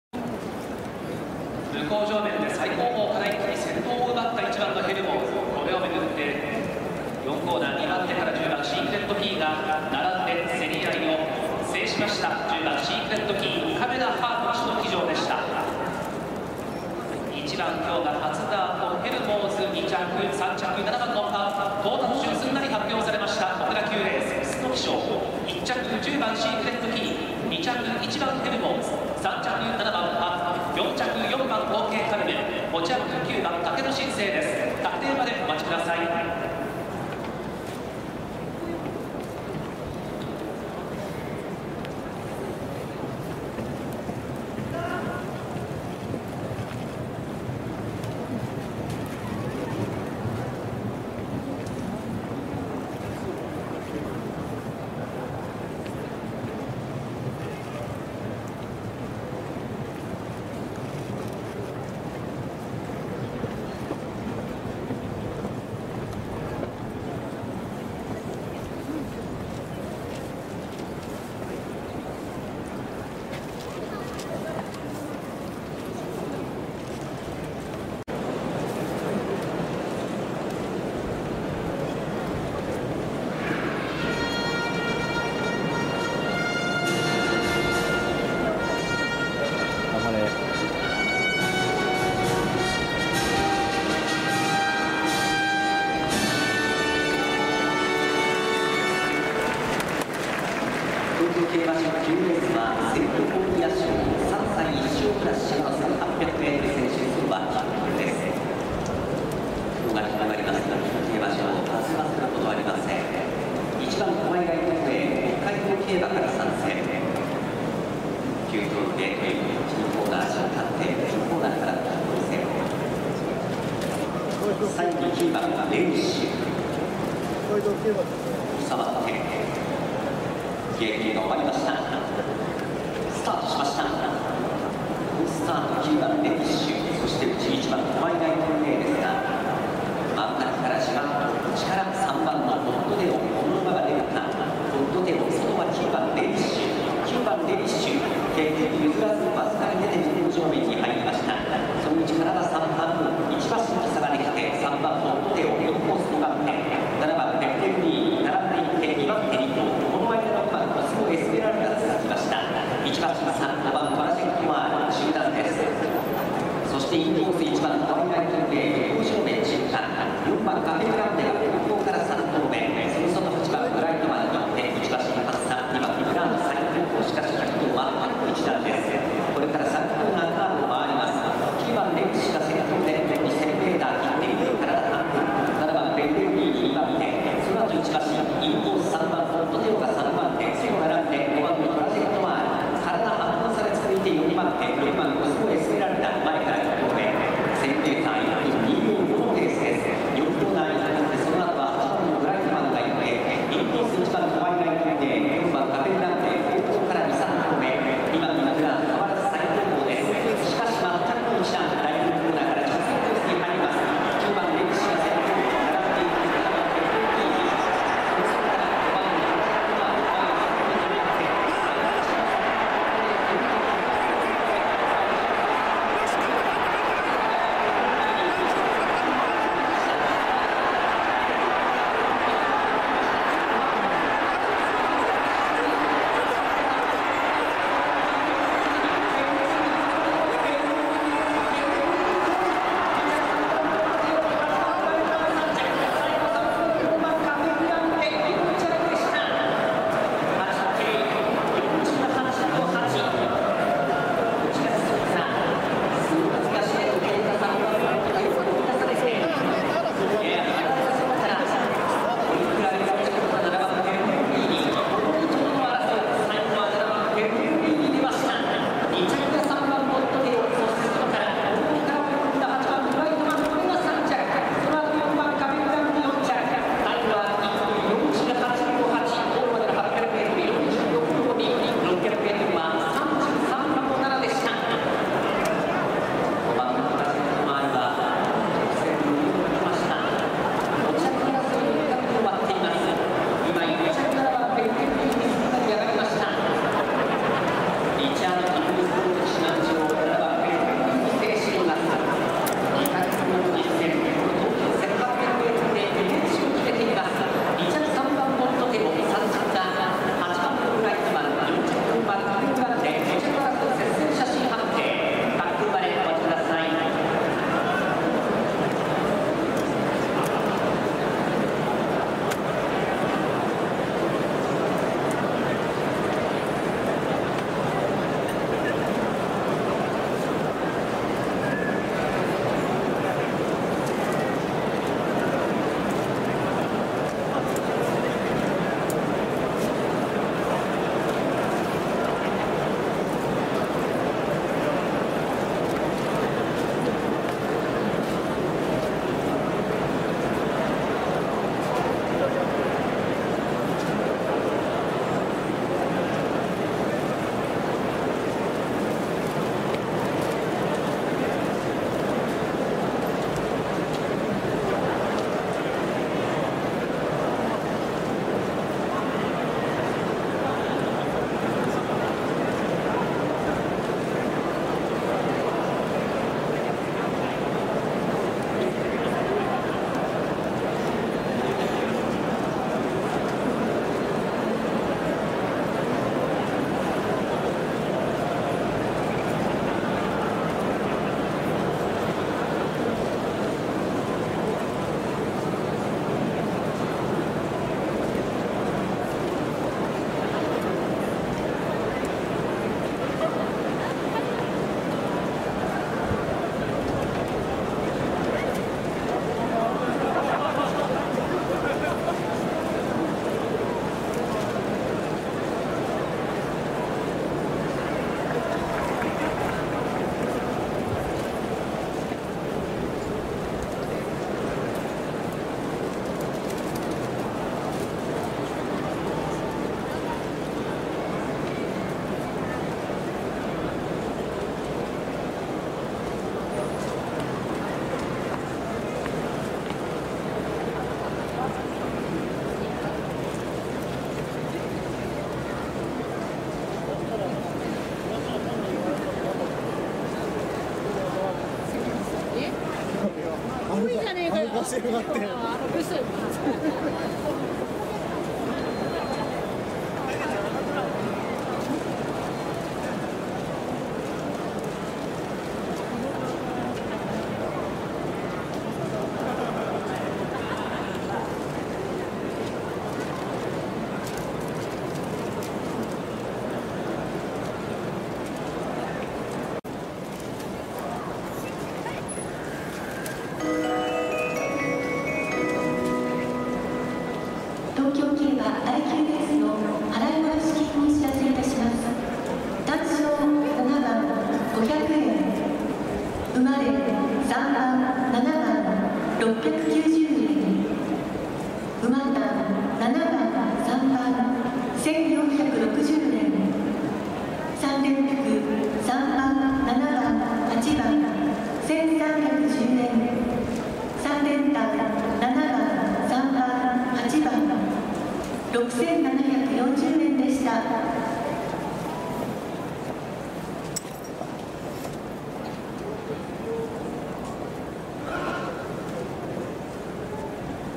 2024 1 28 セントポーリア賞 東京競馬場第９レース トラジェクトワール ポッドテオ ペッレグリーニ 他
ペッレグリーニが勝利したレースを馬場入場から確定までを編集しました